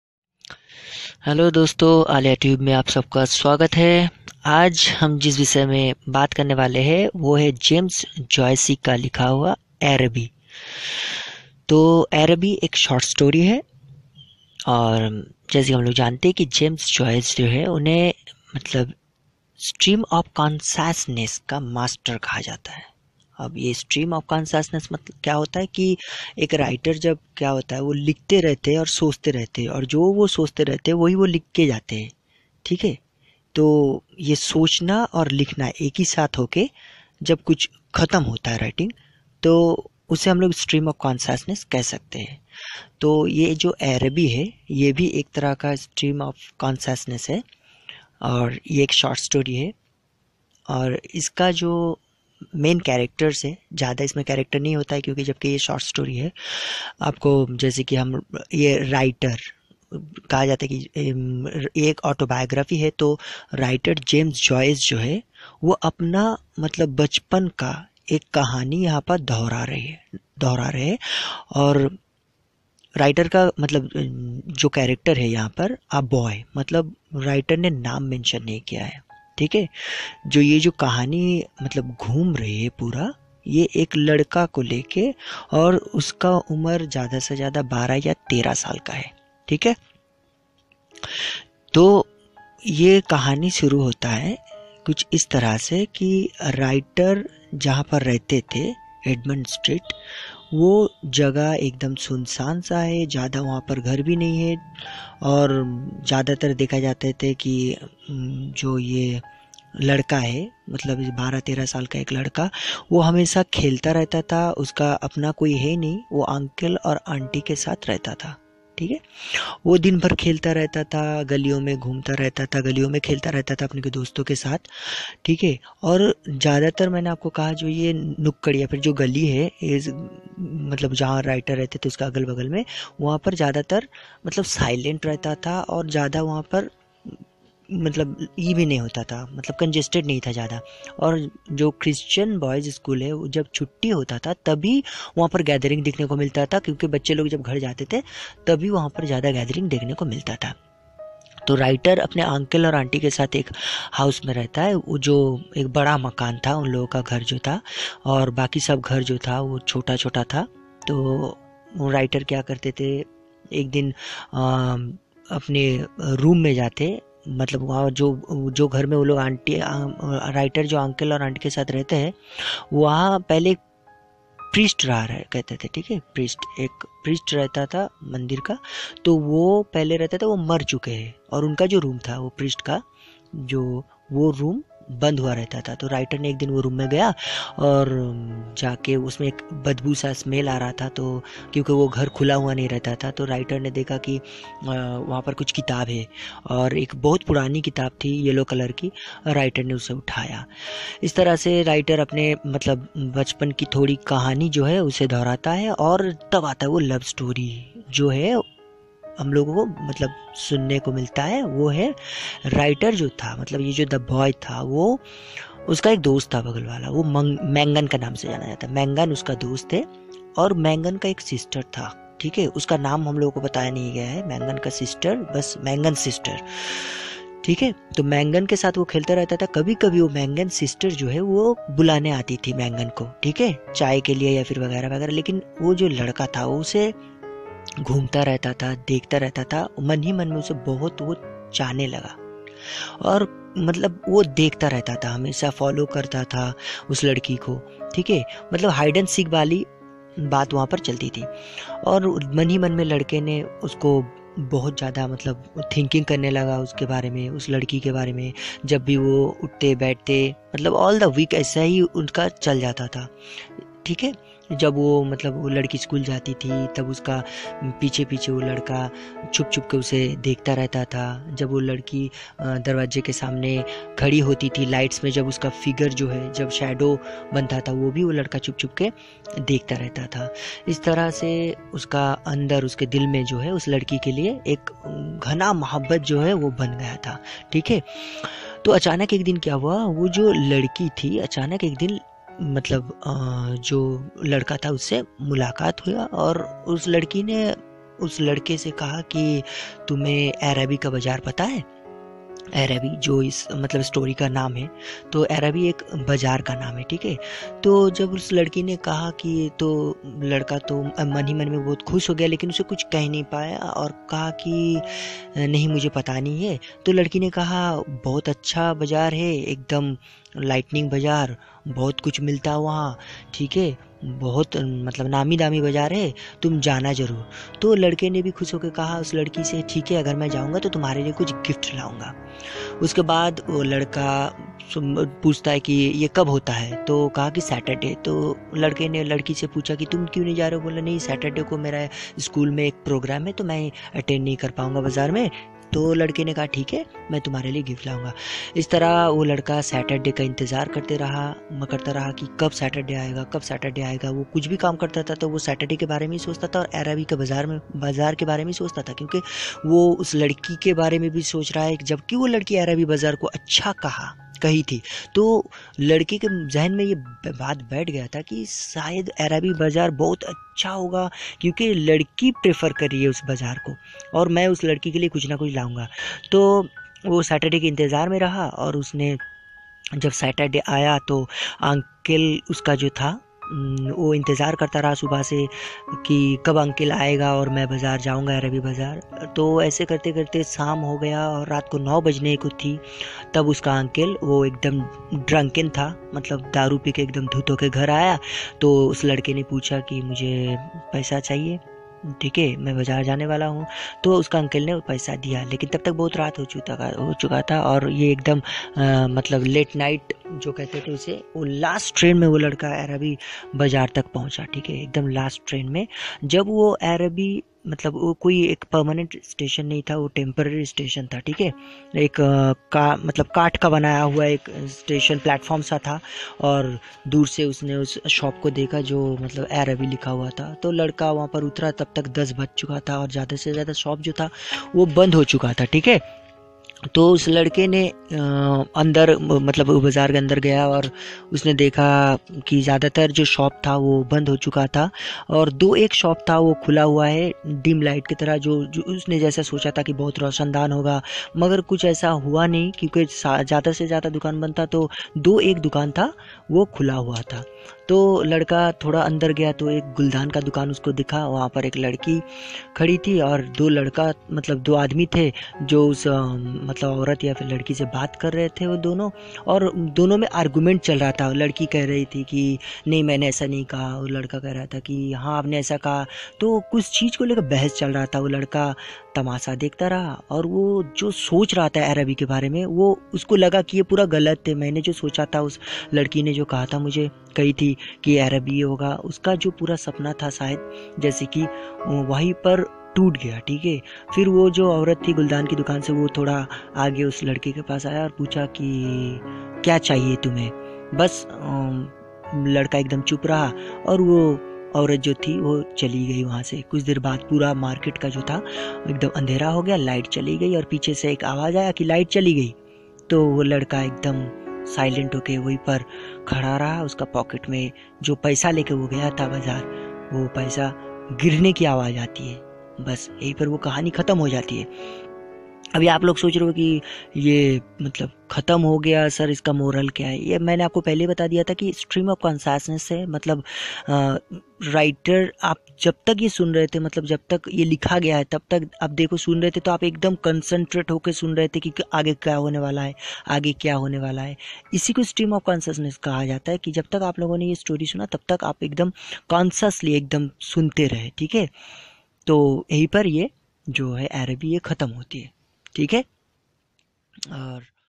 "ARABY" easiest summary and full story in hindi
Araby,a short story by James Joyce